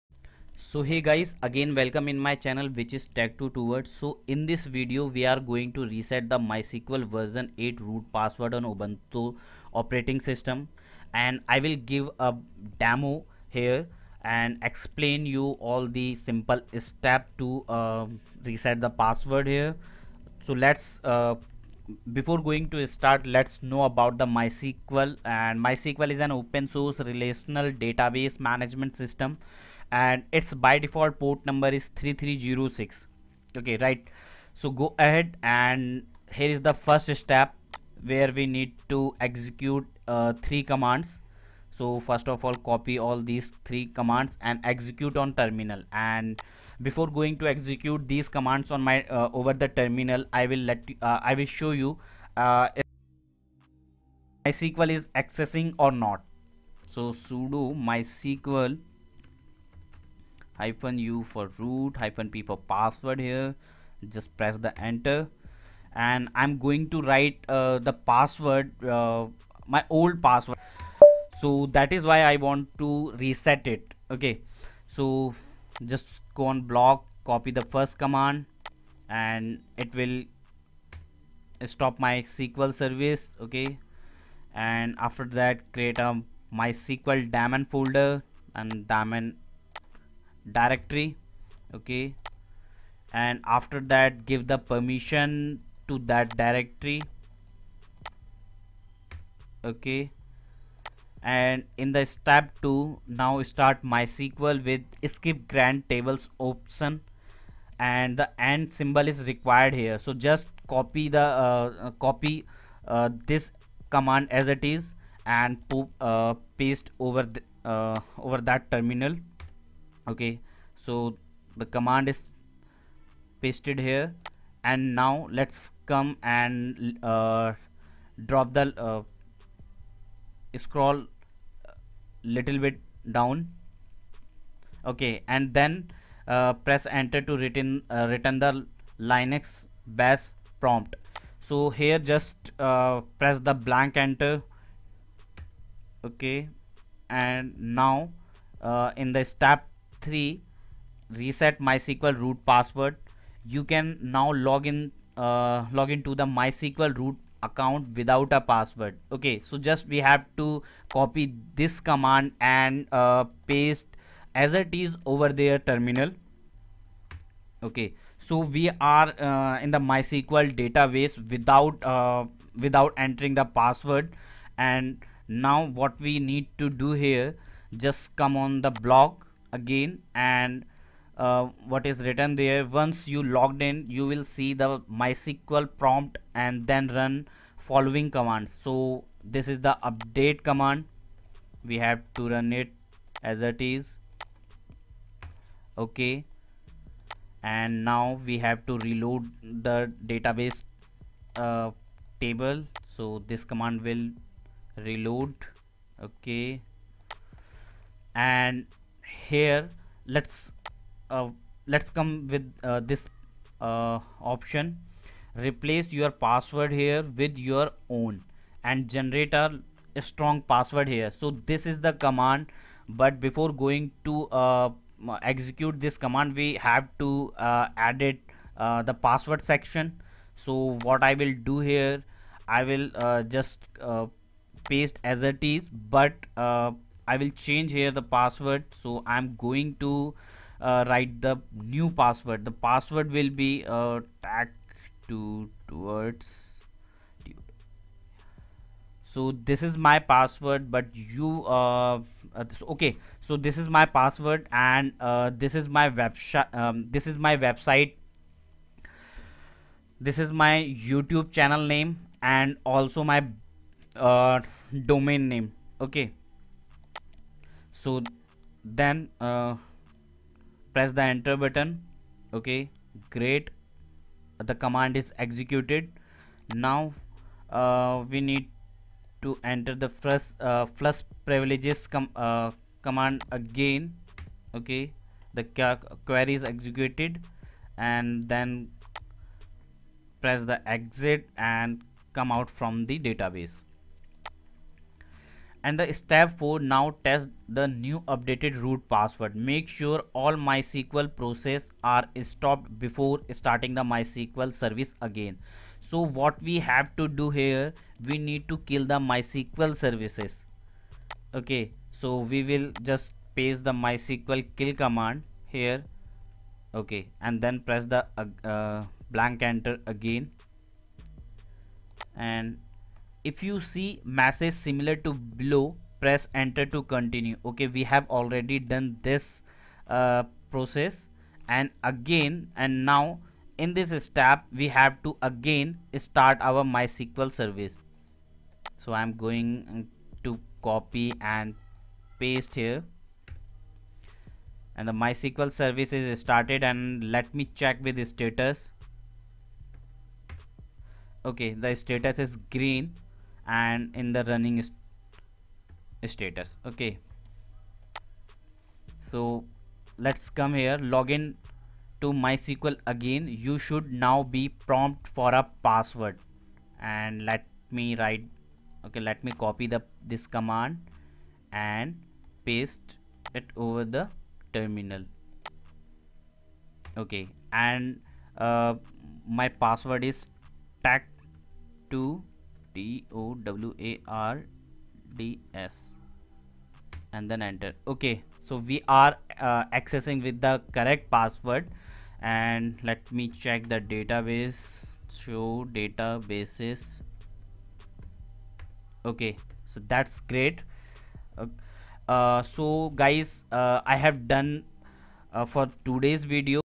How To Reset Mysql 8 Root Password | Reset Mysql Root Password On Ubuntu 20.04/22.04 LTS
In this video we are going to rest mysql 8 root password on ubuntu 20.04 LTS